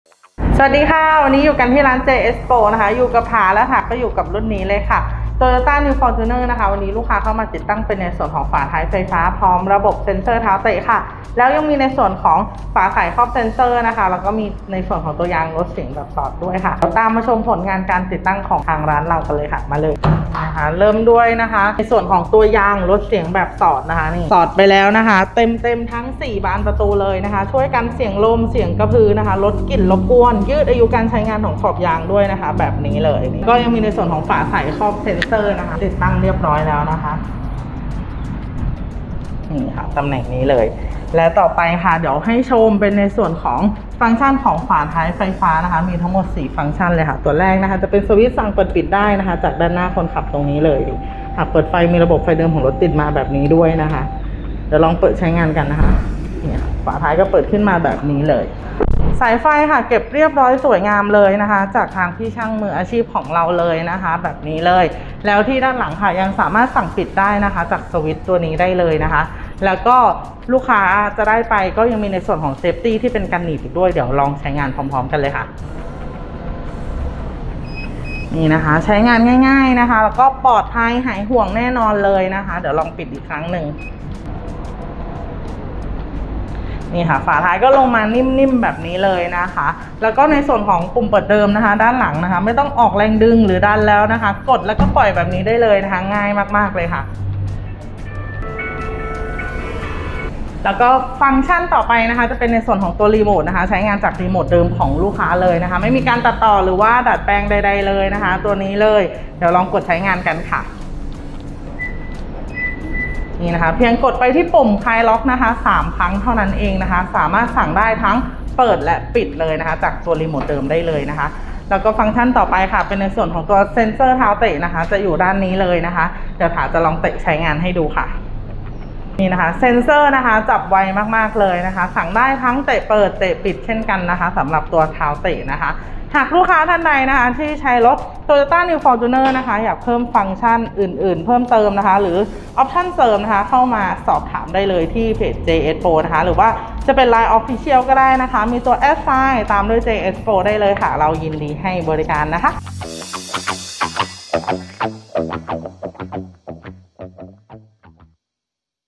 FORTUNER ผลงานการติดตั้งฝาท้ายไฟฟ้าตรงรุ่นพร้อมระบบเซ็นเซอร์เท้า - JSPRO
👉สนใจติดตั้งหรือสอบถามข้อมูลเพิ่มเติม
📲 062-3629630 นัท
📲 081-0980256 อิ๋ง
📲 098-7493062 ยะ
📨Line official id : @jspro (อย่าลืมใส่ตัว@นะครับ)